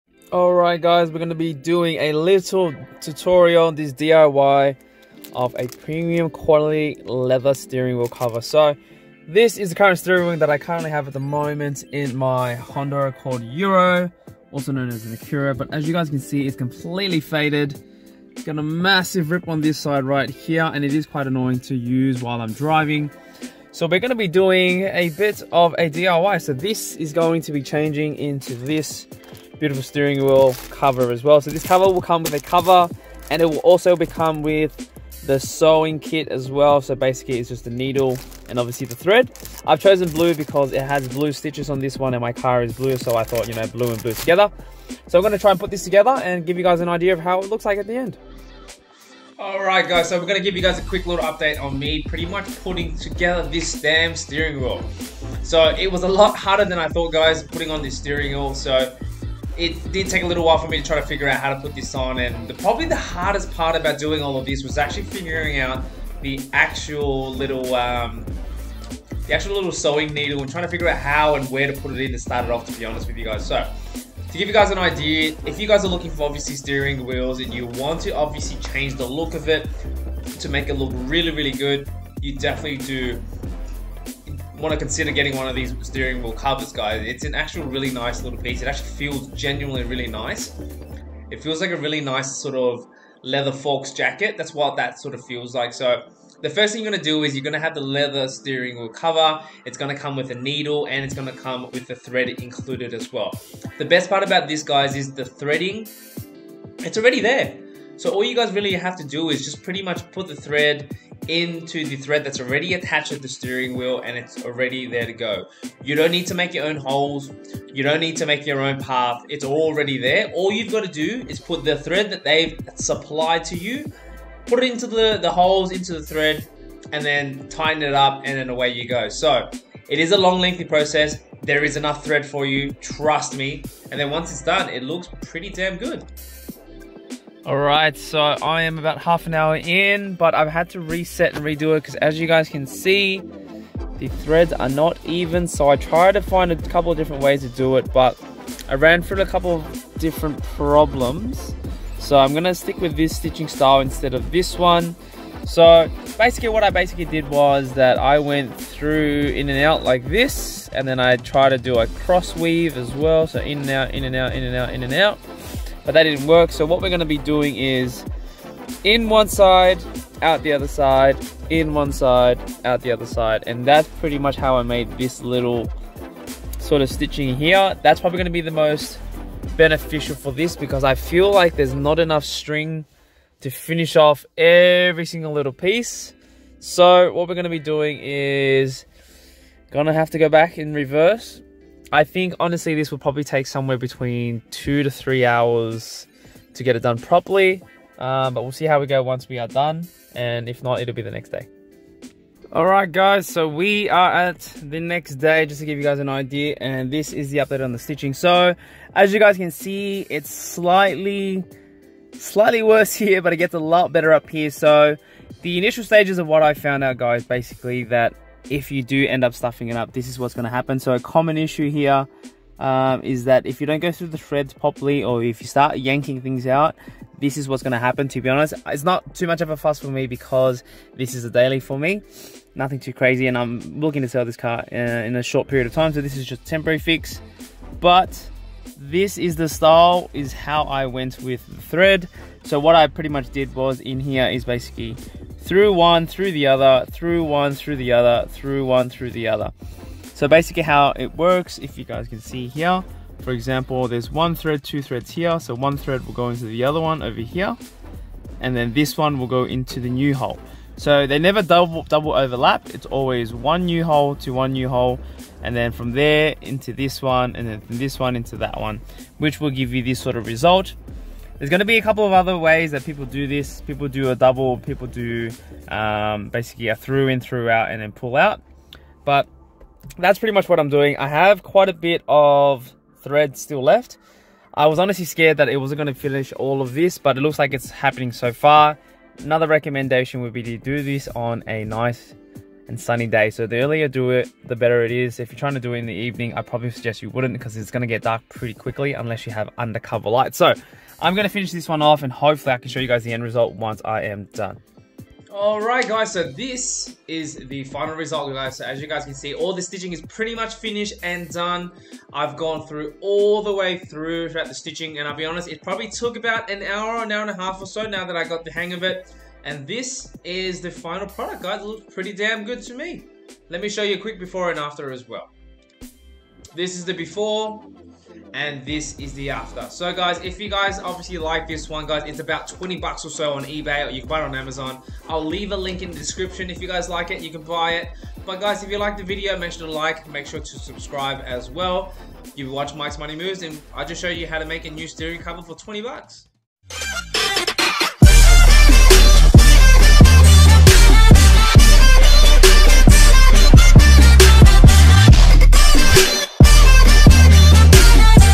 DIY Leather Steering Wheel Restitching for ONLY $20 !
Is your steering wheel looking old? tired? dull?

Want to get a new feel and look but don't want to have the hassle of replacing it which can cost $$$ or having to remove the entire steering wheel?

Here's a cheap and quality look finish on how to make it look better again!
I went through the option of looking for a replacement. I didn't want to replace the entire steering wheel or even get those steering wheel covers which don't exactly fit or feel nice.

So i thought why not try one of these restitching covers and try it for myself. Honestly it feels amazing and like it came straight from the factory. Look, it's not going to be brand new new but it's the next best thing and i can honestly say i am very happy with it.